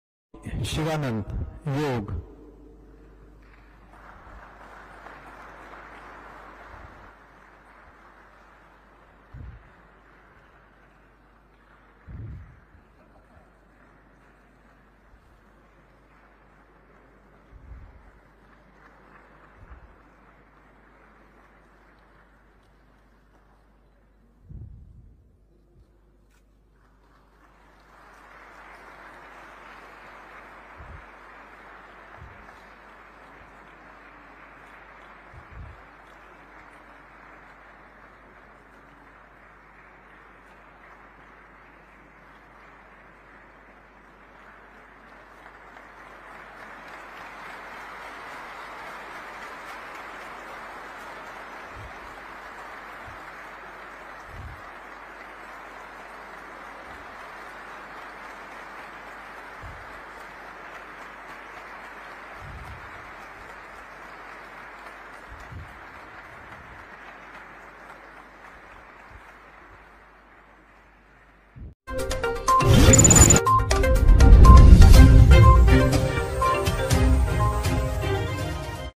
126 साल के स्वामी शिवानंद को पद्मश्री पुरस्कार से किया गया सम्मानित...
योग के लिए राष्ट्रपति रामनाथ कोविंद ने किया सम्मानित...
वाराणसी के है 126 साल के स्वामी शिवानंद .